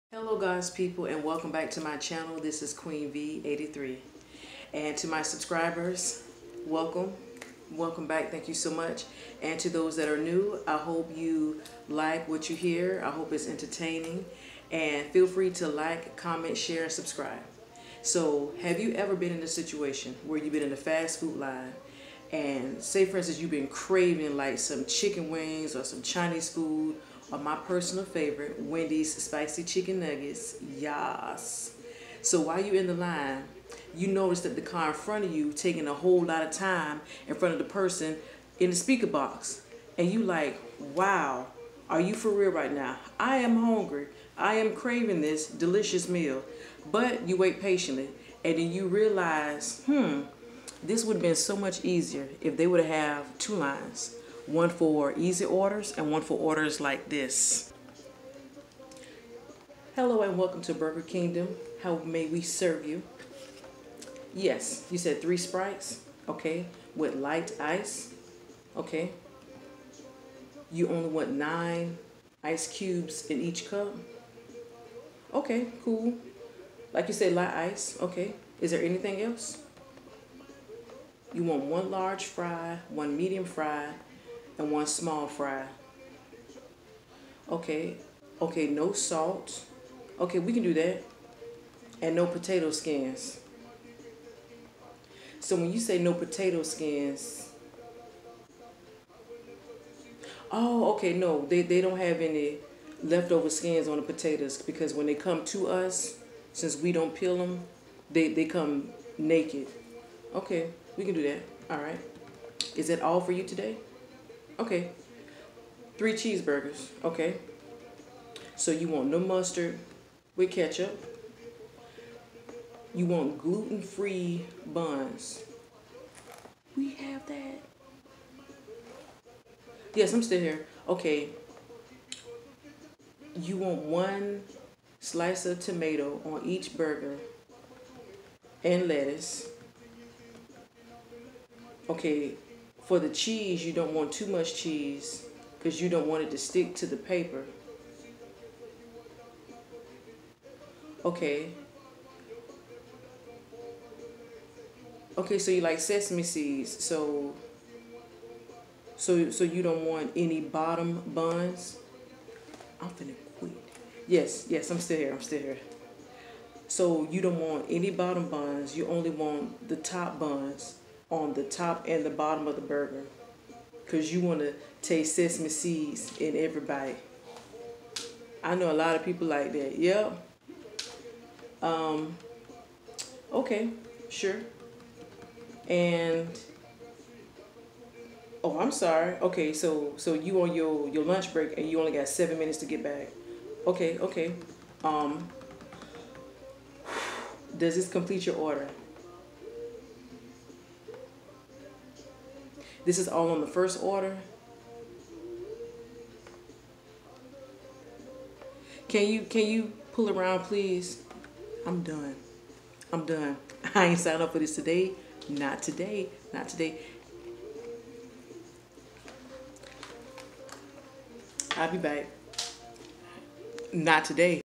ItsComplicated
Don't you just HATE it when you go to get something quick from your fav fast food spot and this happens!!!
Btw, shout out to the folks serving fast food!!! Thank you 👍😎😊